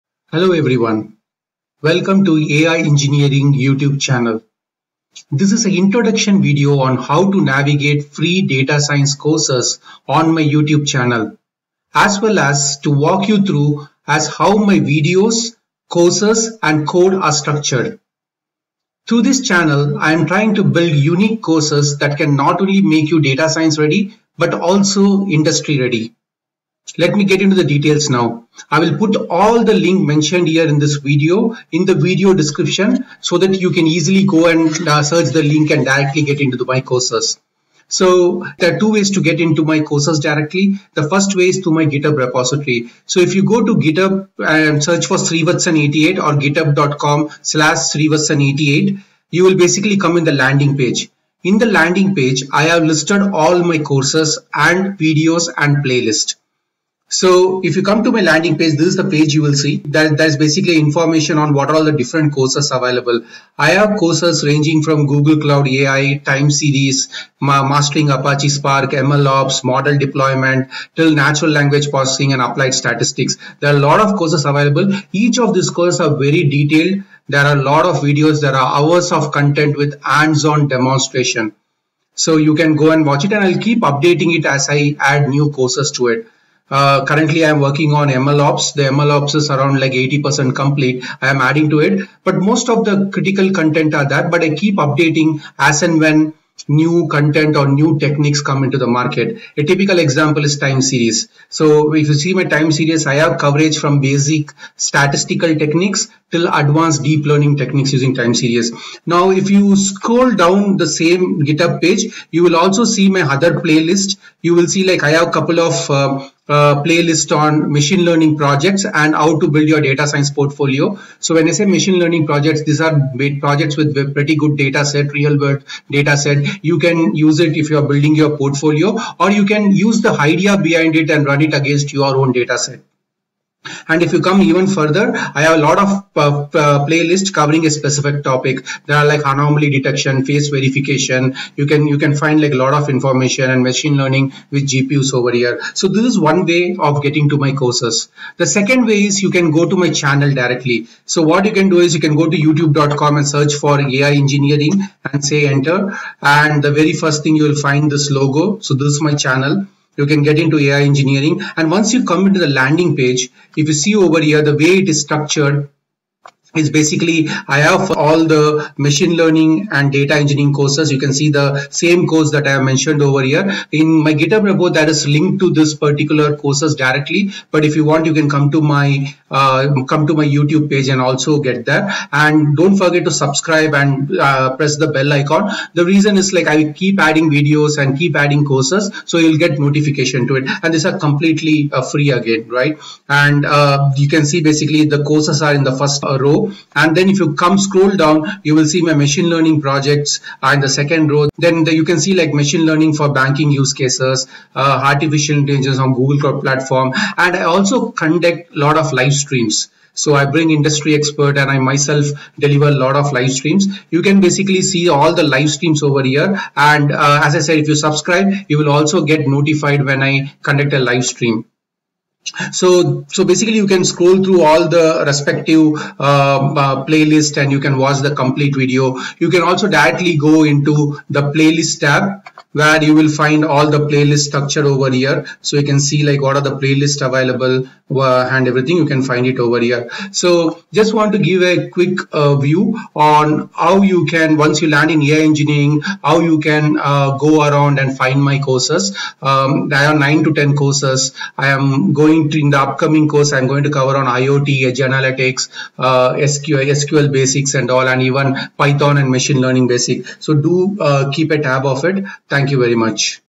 Free data science and machine learning courses - AIEngineering
This video is a quick introduction to courses, content and code in my channel AIEngineering. This provides a guide to navigate through my channel content